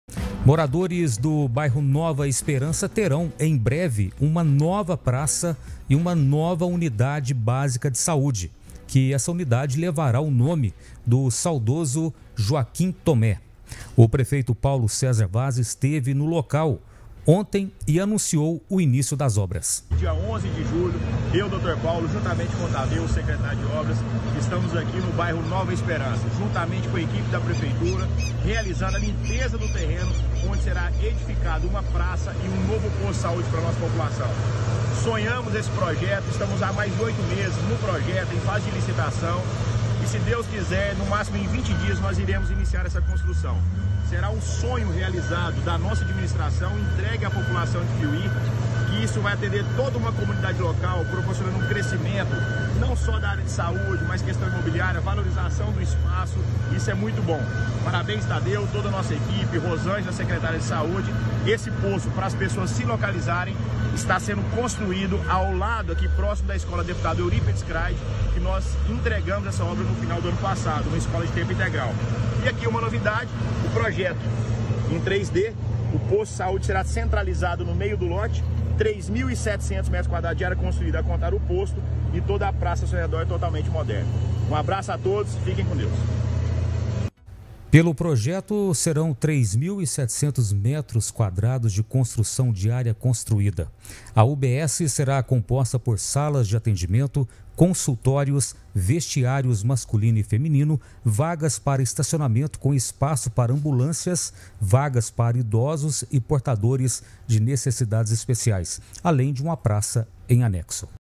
PREFEITURA DE PIUMHI INICIA CONSTRUÇÃO DE PRAÇA E UBS JOAQUIM TOMÉ
| PIUMHI |

MORADORES DO NOVA ESPERANÇA TERÃO, EM BREVE, UMA NOVA PRAÇA E UMA UNIDADE BÁSICA DE SAÚDE, QUE LEVARÁ O NOME DO SAUDOSO JOAQUIM TOMÉ. O PREFEITO PAULO CÉSAR VAZ ESTEVE NO LOCAL ONTEM E ANUNCIOU O INÍCIO DAS OBRAS. VEJA: